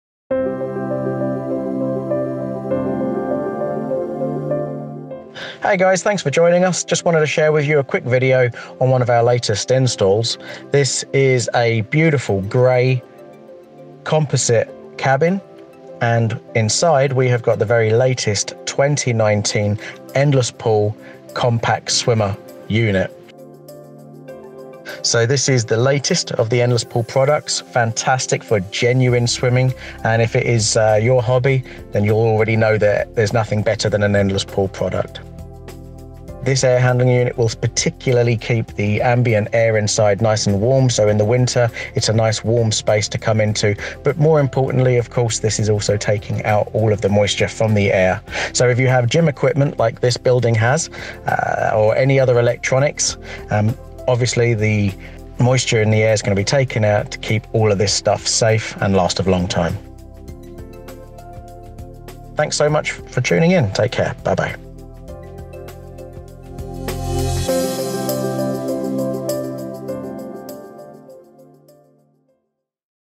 Endless Pools
Modern grey composite built lodge with 2019 Endless Pool Streamline Compact Swimmer